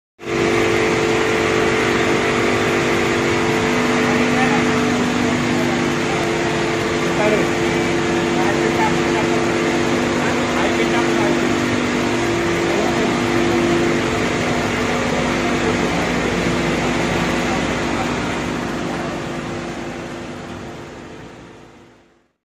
Bird net for Factory louvers.
Humane and effective bird net installation for manufacturing plants.